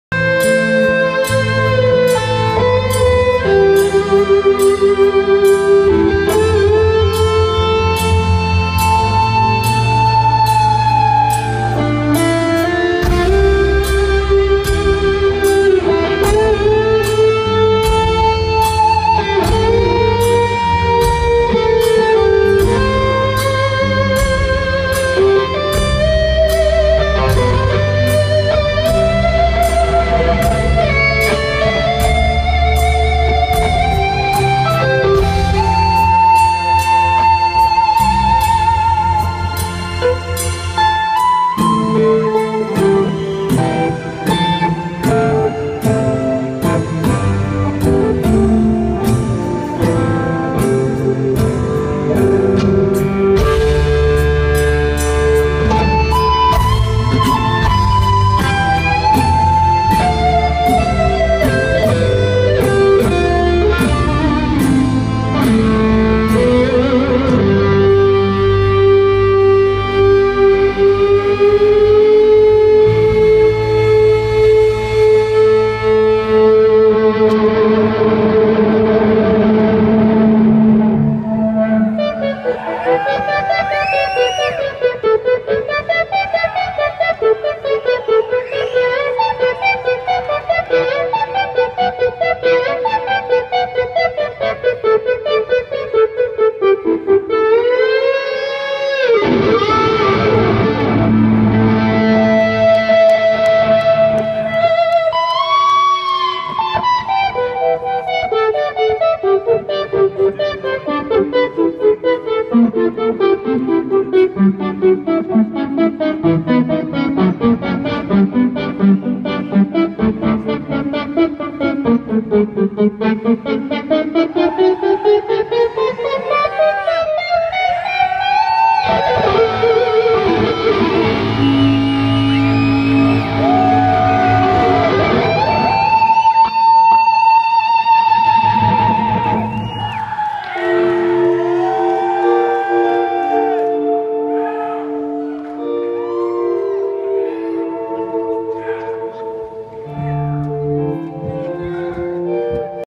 Steve Morse- Grand Prairie, TX
Deep Purple at the Verizon Theater in Grand Prairie, TX. Video was cut short thanks to that asshole who covered the camera on purpose.